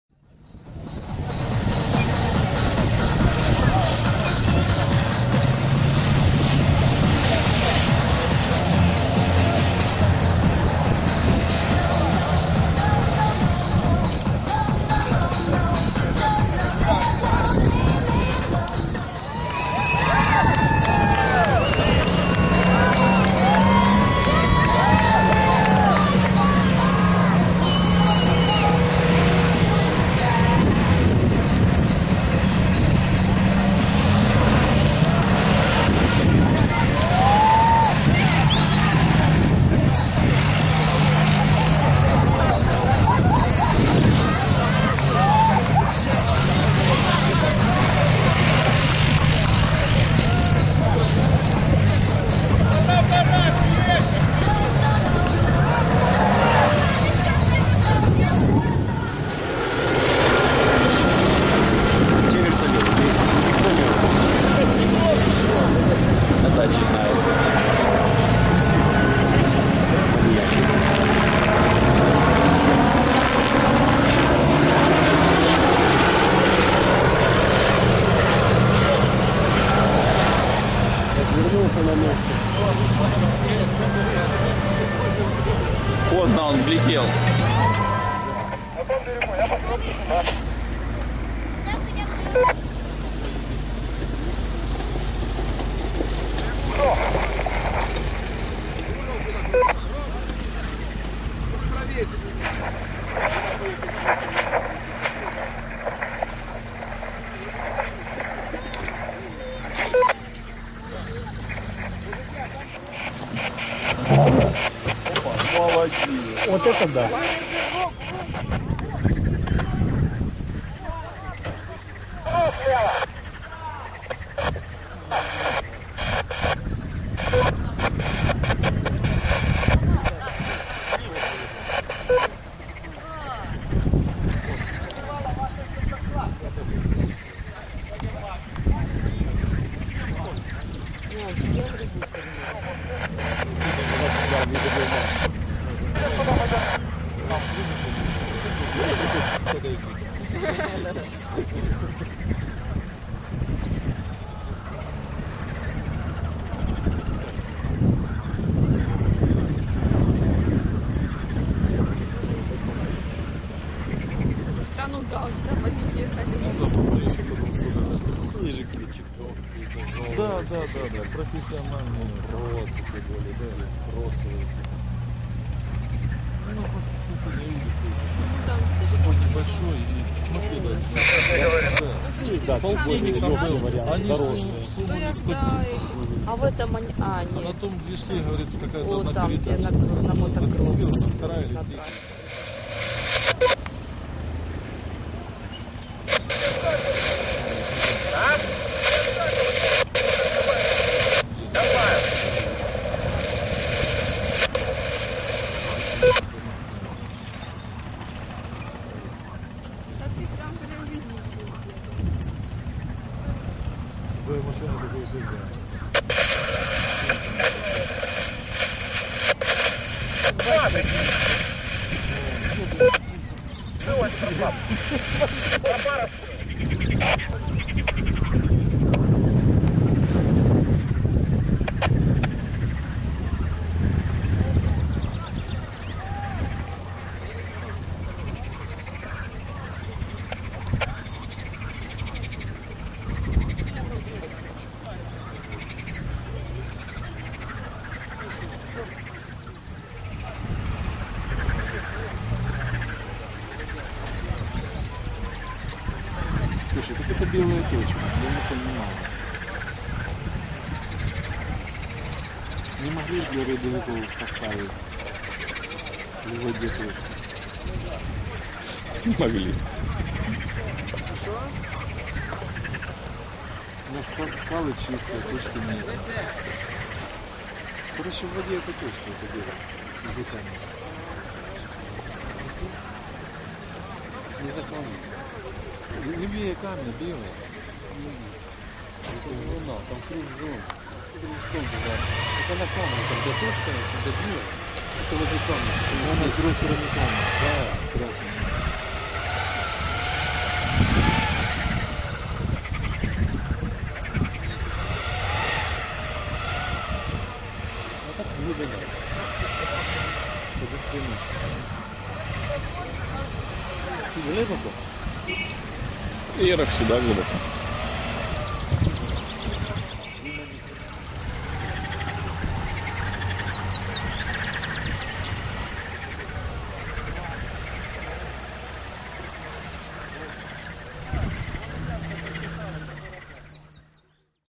Барсук Трофи `11 Часть 3: В карьере
Барсук трофи, Кременчуг, 29 апреля - 01 мая 2011
Второй этап кубка Восточной Украины по трофи-рейдам